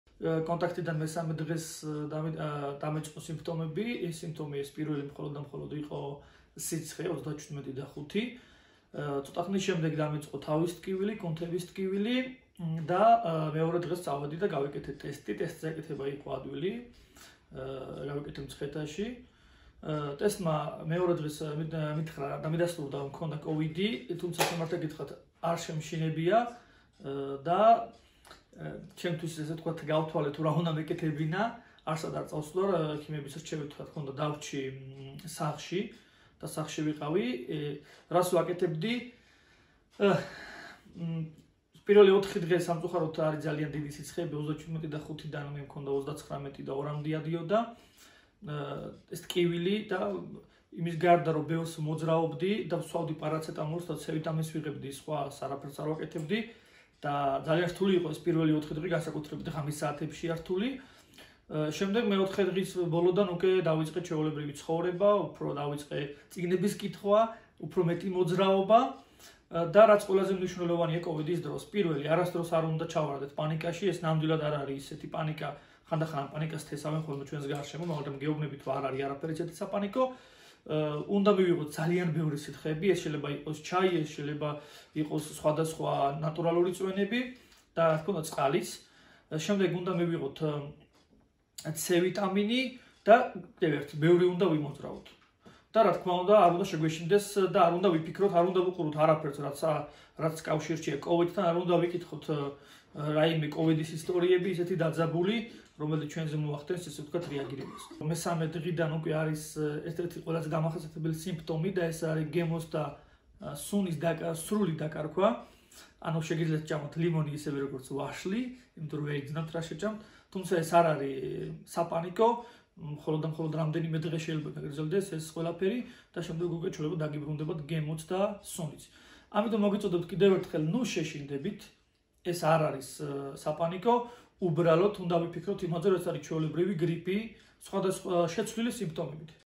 ‘პანიკაში არ უნდა ჩავვარდეთ’ – კოვიდინფიცირებულის გამოცდილება
კოტე აკოფაშვილმა კორონავირუსი, სახლში 12 დღიანი მკურნალობის შემდეგ დაამარცხა. ტესტიც აიღო, რომელმაც უჩვენა, რომ კოვიდუარყოფითია.